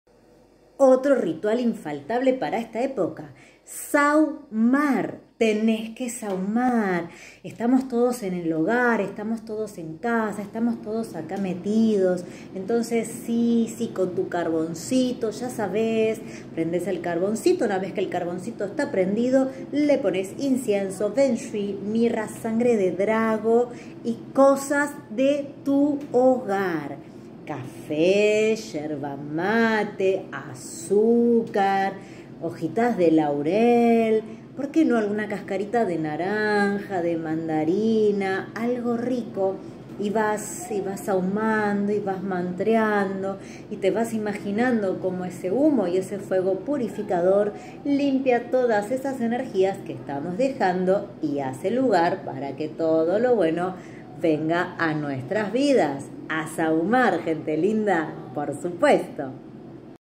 Más rituales para este solsticio de invierno 2022
❄️Rituales🍂
✨ Solsticio de Invierno
🤲🏽Está noche más oscura del año nos invita
🔥A  sahumar,prende el carboncillo y cuando se prende le pones, con incencio, Benjui, sangre de Drago, mirra y cosas que tengas en tu hogar 🏡azúcar, yerba, Cafe, lo que tengas ✨
🪄En estos días vamos a estar subiendo mucha info
🔥¿Con que sahumar?